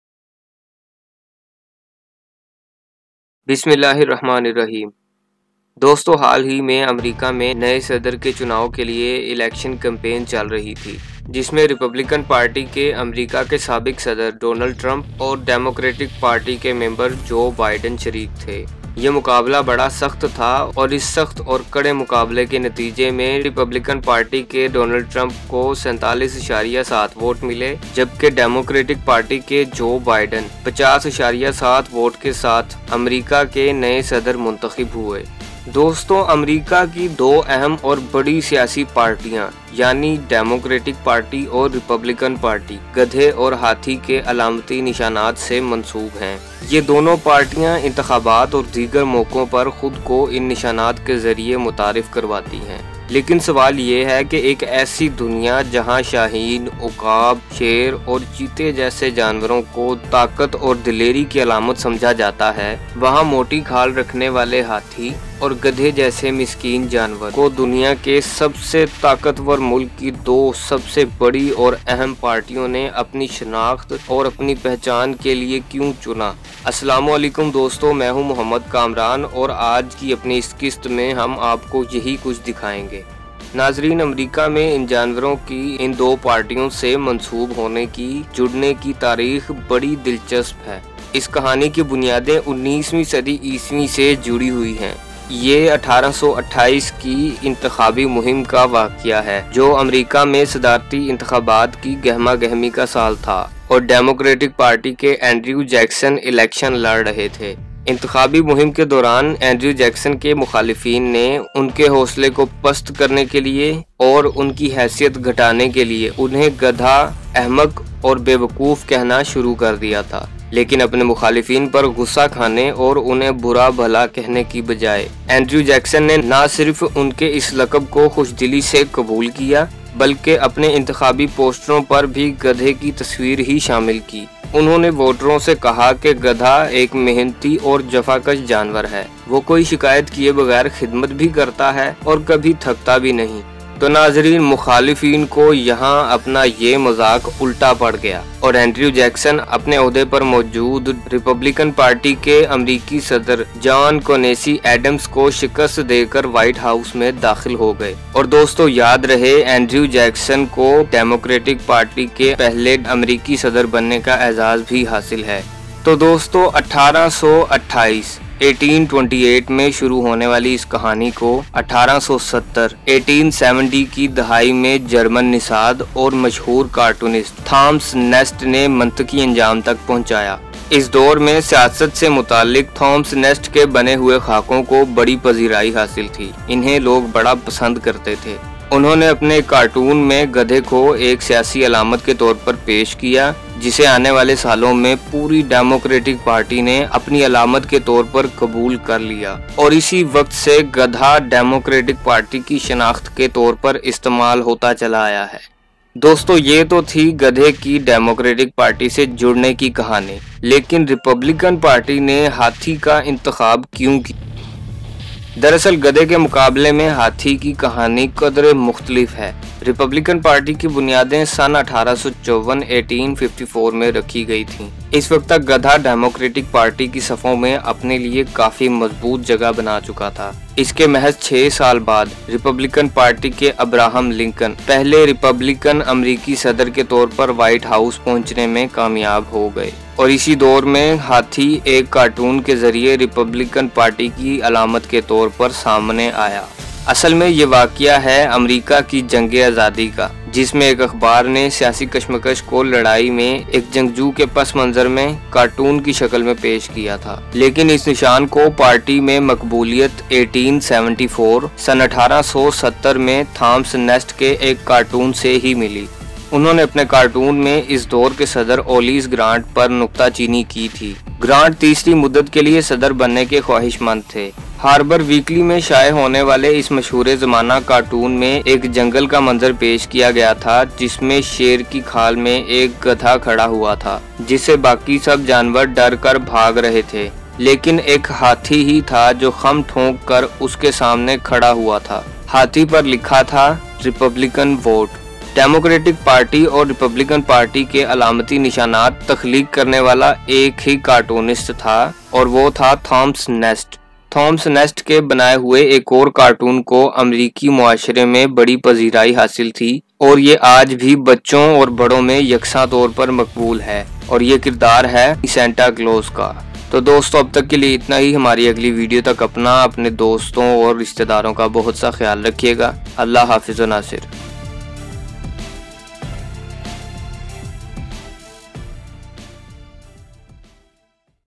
History Of Democratic Vs Republican Party Sign | History Of America
This video based on the history of Democratic party sign Which is Elephant vs Republican party sign which is Donkey. This video lightens some part of  US election campaign in 2020, US election campaign 1828 and 1854.
Joe Biden wins and gave surprise to Pakistan.